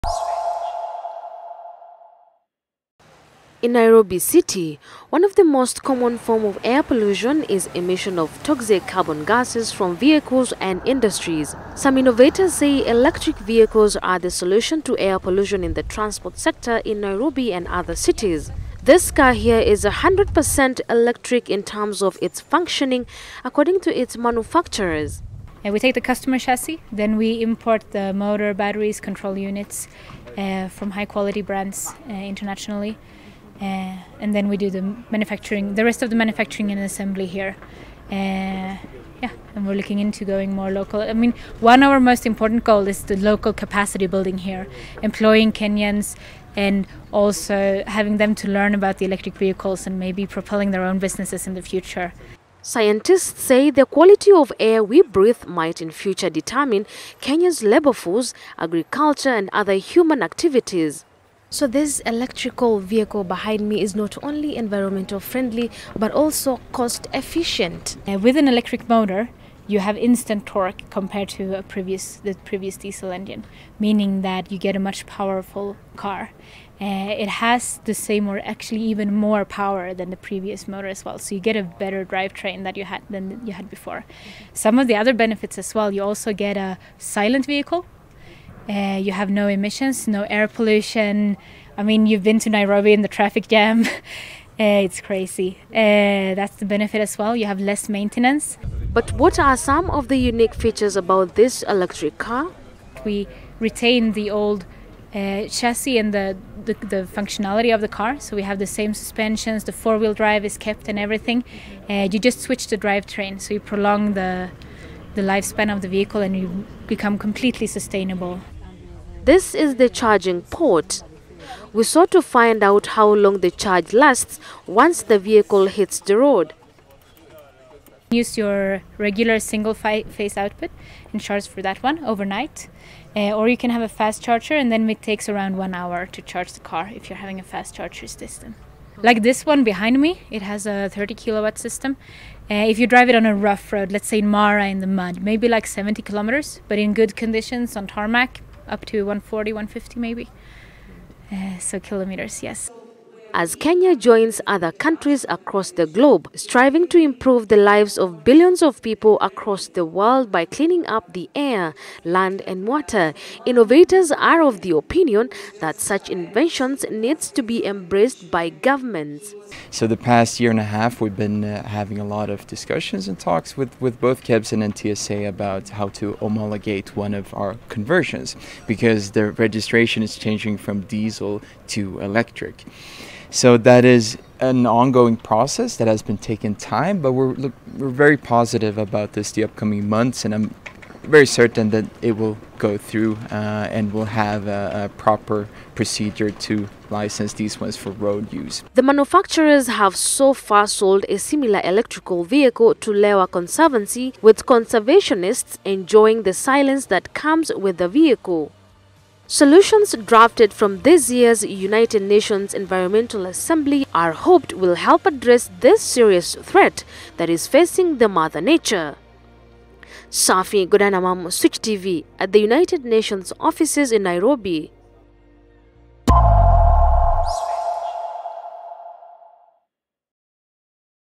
Electric Vehicle Made In Kenya -UNEA CONFERENCE
EXPERTS IN THE TRANSPORT INDUSTRY INTRODUCES 100% ELECTRIC VEHICLES  IN A BID TO CURB AIR  POLLUTION